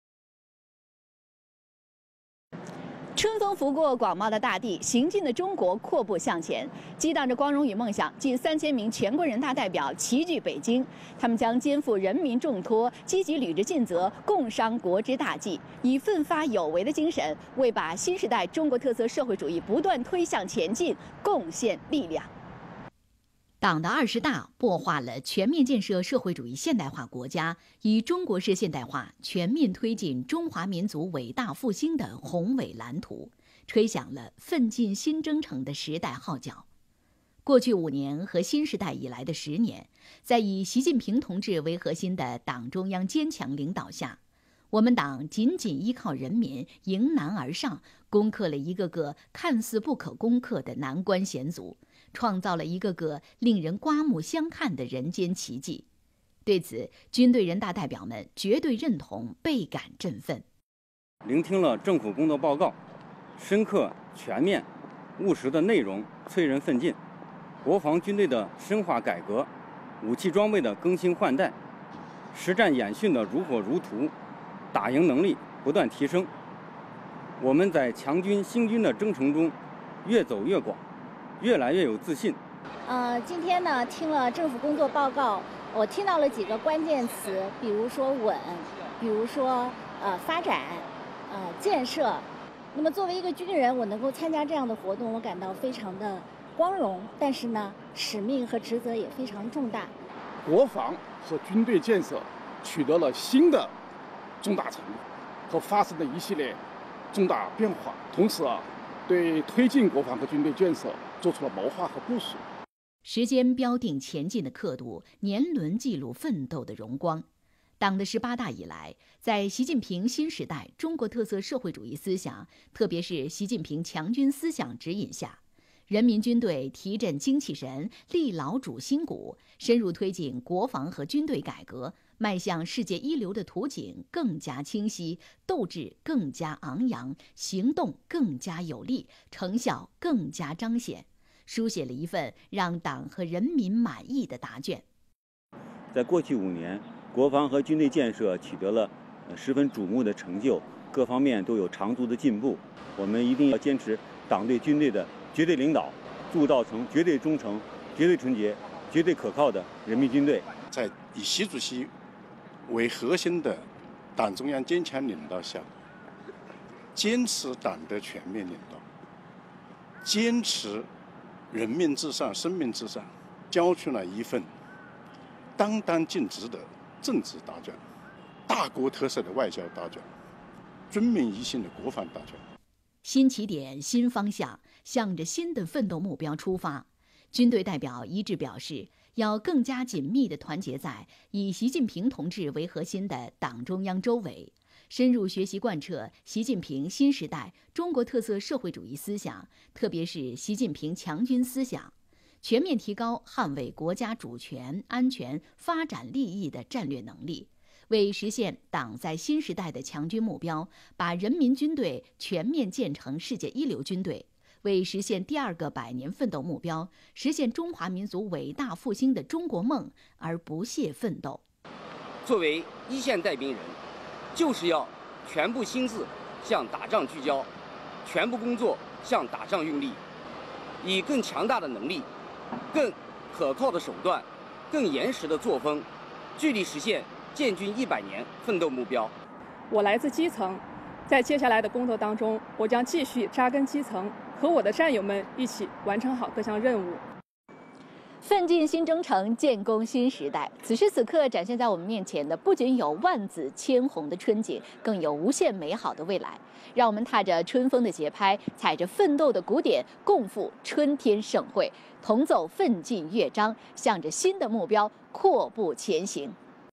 聚焦两会新闻特写：激扬奋进力量创造新的伟业
党的二十大擘画了全面建设社会主义现代化国家、以中国式现代化全面推进中华民族伟大复兴的宏伟蓝图，吹响了奋进新征程的时代号角。过去五年和新时代以来的十年，在以习近平同志为核心的党中央坚强领导下，我们党紧紧依靠人民迎难而上，攻克了一个个看似不可攻克的难关险阻，创造了一个个令人刮目相看的人间奇迹。对此，军队人大代表们绝对认同，倍感振奋。